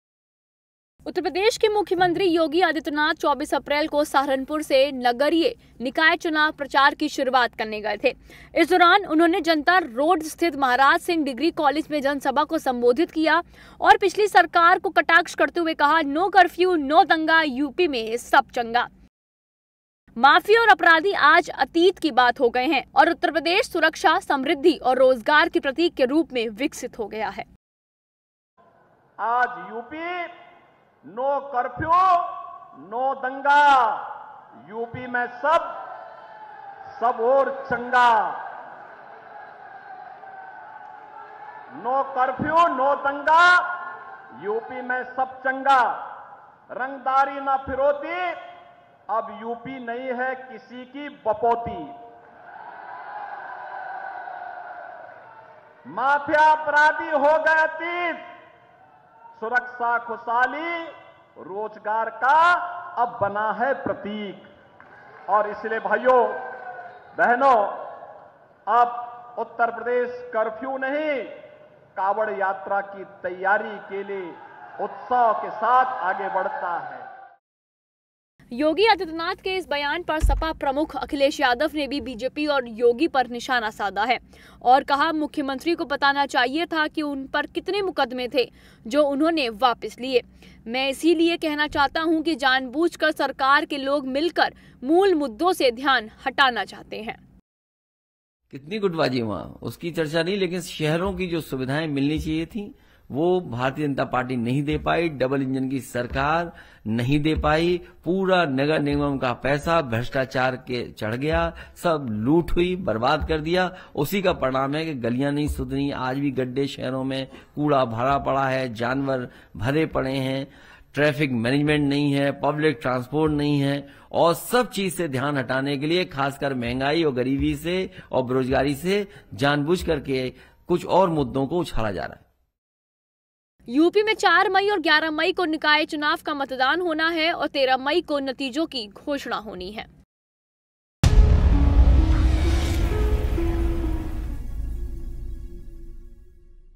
Yogi का Akhilesh Yadav पर निशाना बोले, नो कर्फ्यू-नो दंगा, यूपी में सब चंगा
योगी आदित्यनाथ ने कहा नो कर्फ्यू-नो दंगा, यूपी में सब चंगा. माफिया और अपराधी आज अतीत की बात हो गए हैं और उत्तर प्रदेश सुरक्षा, समृद्धि और रोजगार के प्रतीक के रूप में विकसित हो गया है.

 Yogi Adityanath said no curfew-no riots, everything is fine in UP. Mafia and criminals have become a thing of the past and Uttar Pradesh has developed as a symbol of security, prosperity and employment.